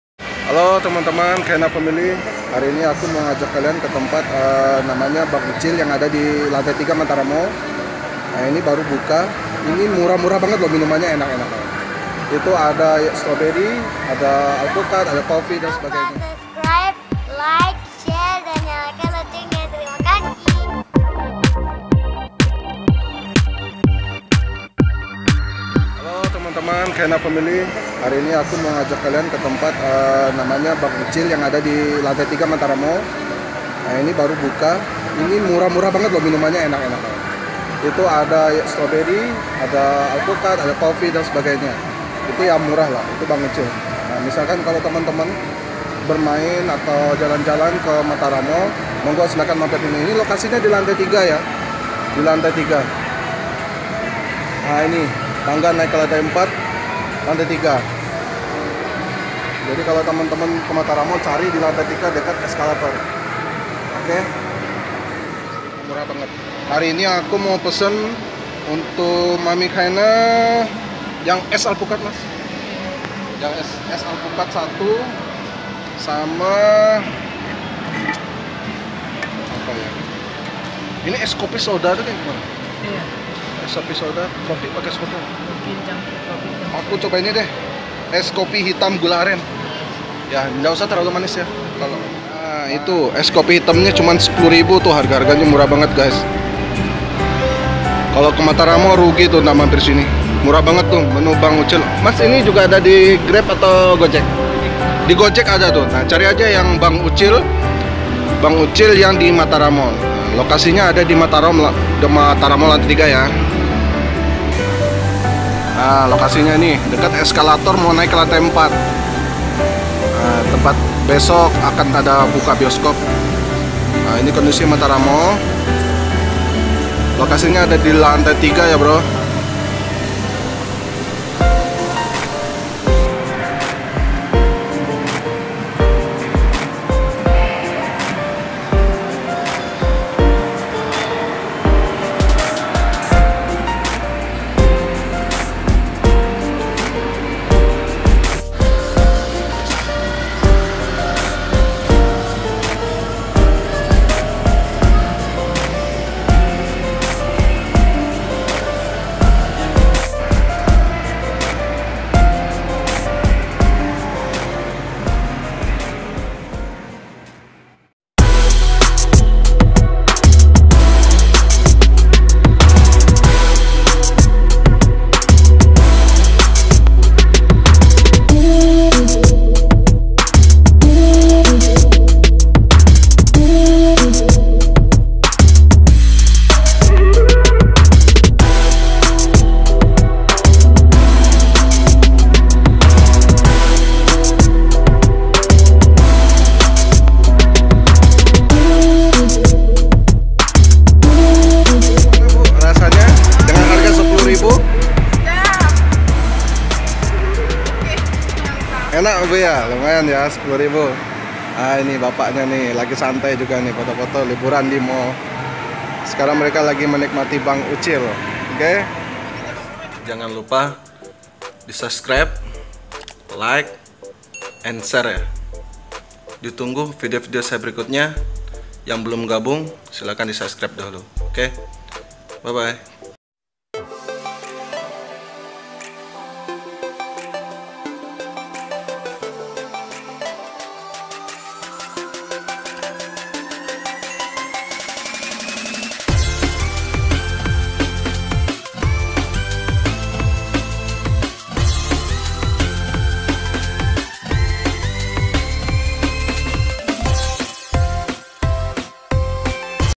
Cobain Minuman Bang Ucil Lombok
Yuk..yang mampir jalan jalan ke mataram mall.nah di lantai 3 mall ada penjual minuman segar baru buka...namanya Bang Ucil ..yuk mampir ya harga murah murah loh.